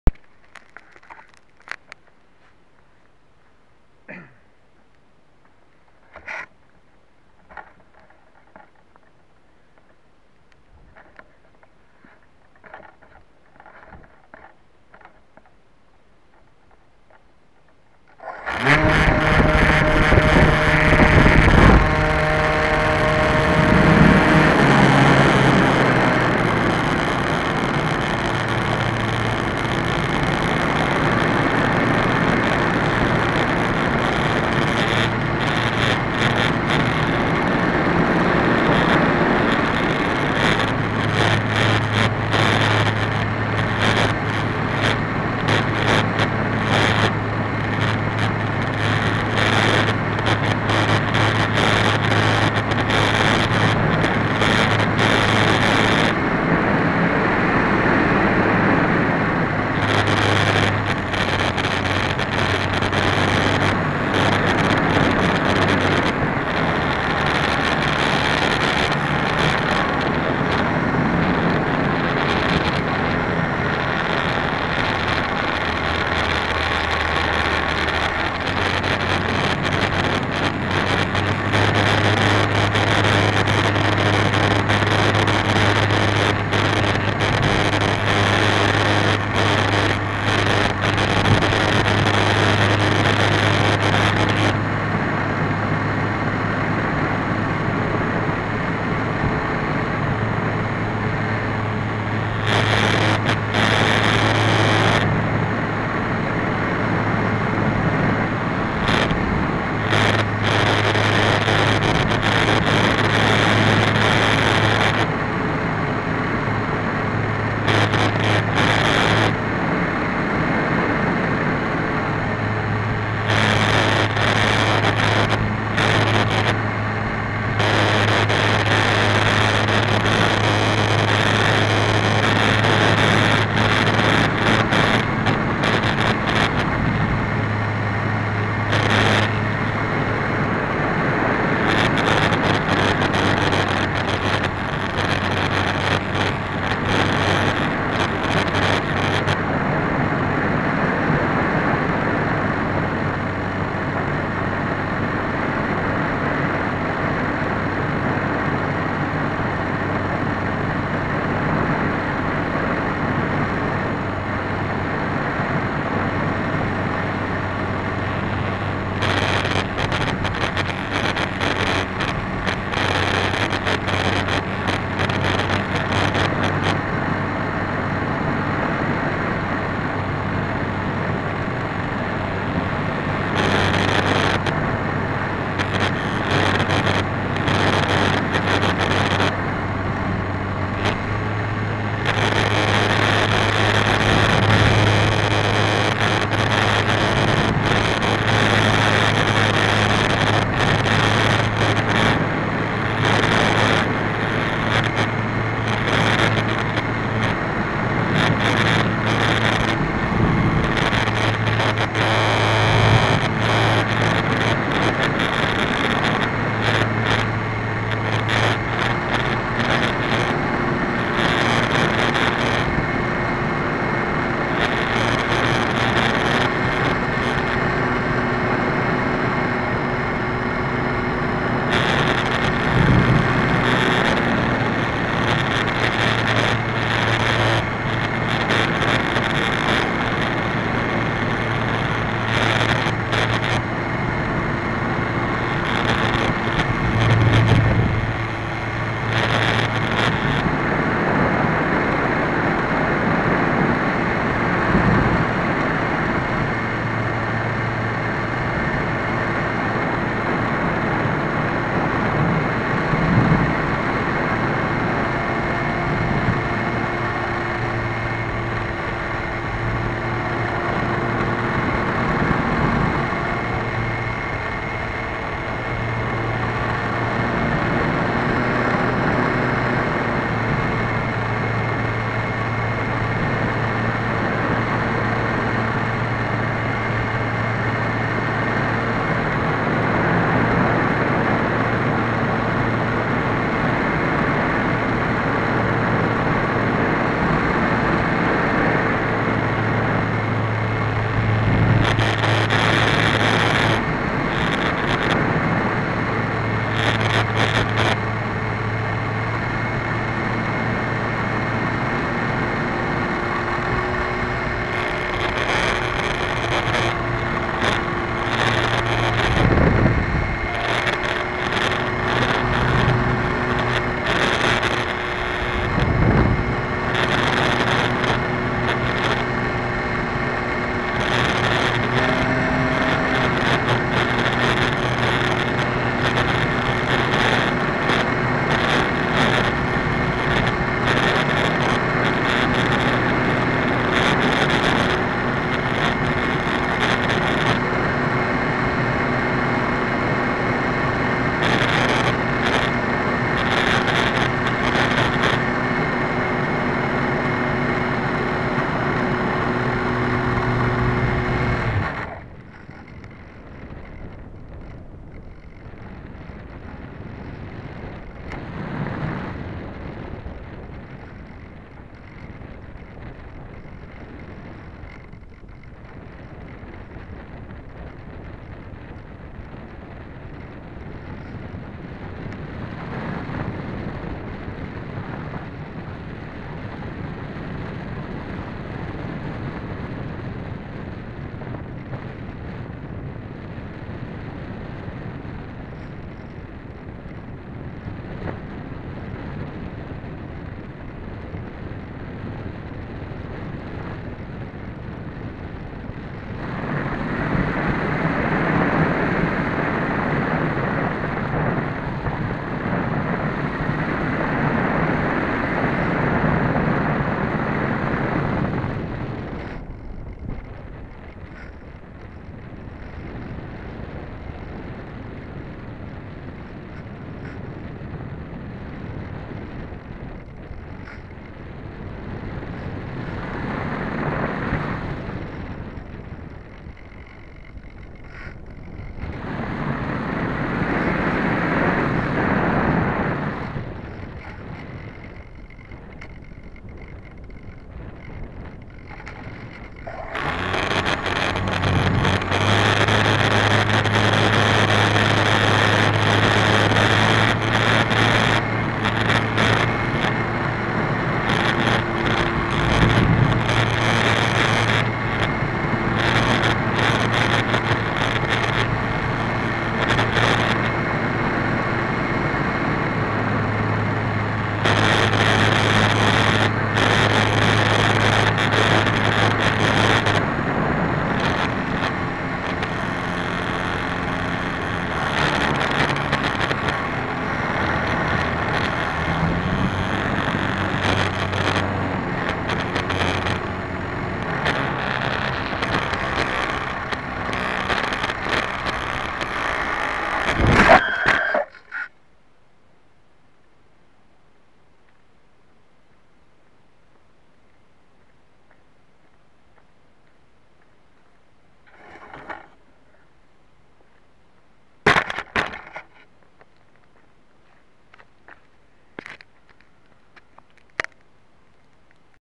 avion rc électrique
avion rc électrique avec micro caméra crach à atterrissage